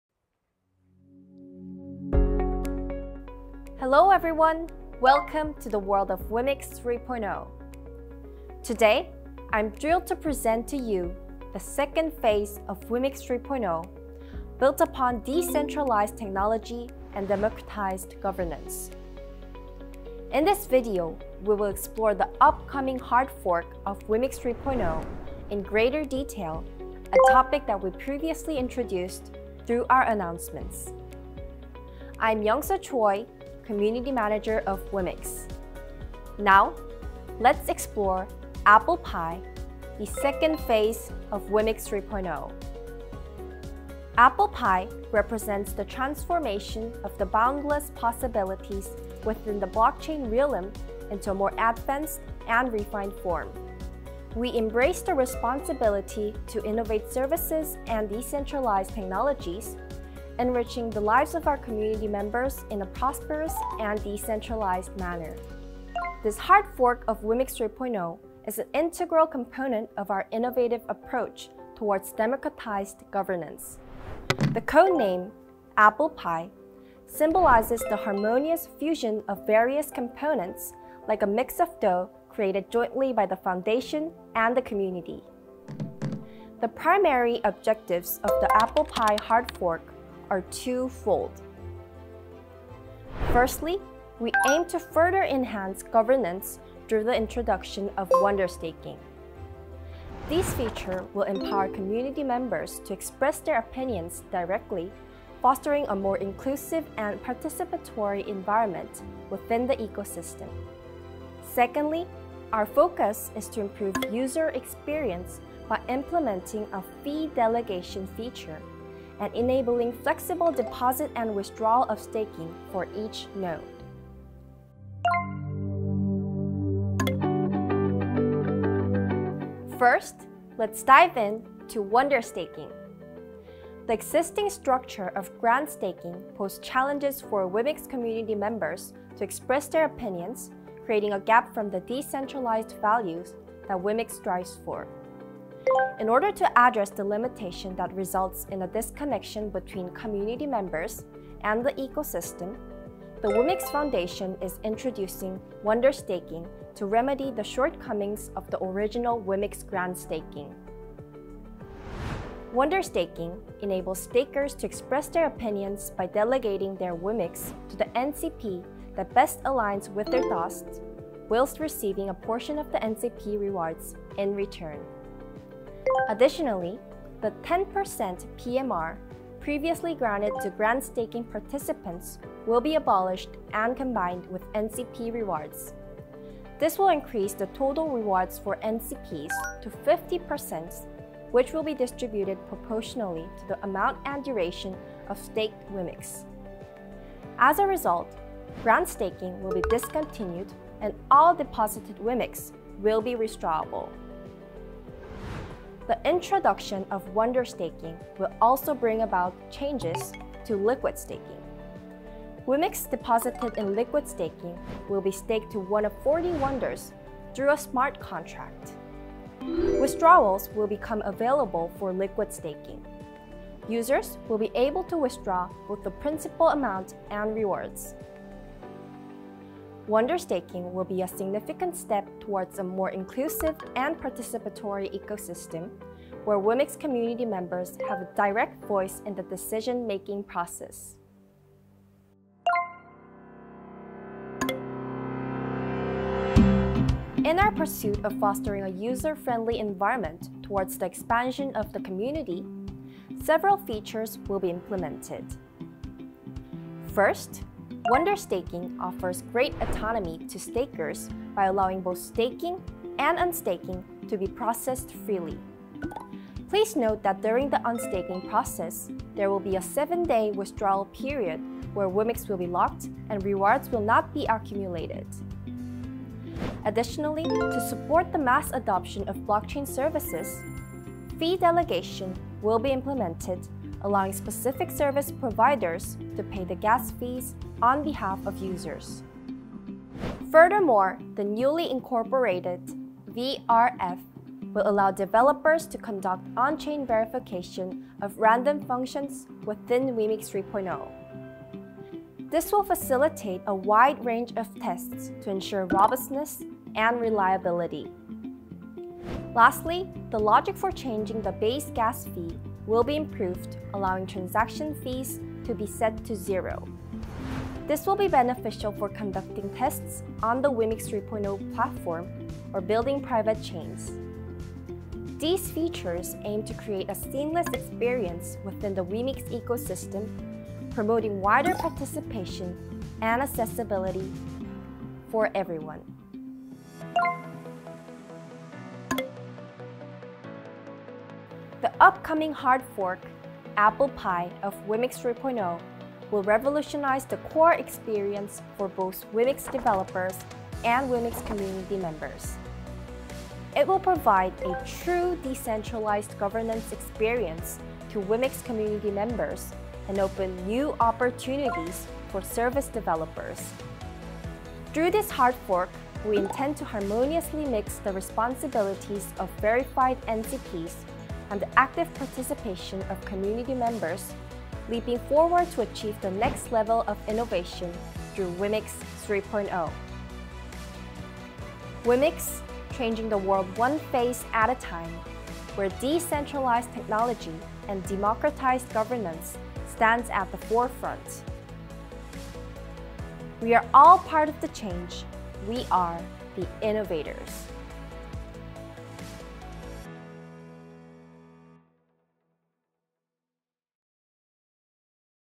[WEMIX3.0] Introducing Applepie - Phase02 of WEMIX3.0 : Birth of Wonder Staking
In WEMIX 3.0 Phase 2, the foundation aims to upgrade the governance system to achieve a more democratic ecosystem and innovate decentralized technologies. Users can contribute to the innovation of the WEMIX 3.0 ecosystem by participating in WONDER Staking, where users delegate WEMIX to 40 WONDERS, and taking advantage of newly added features.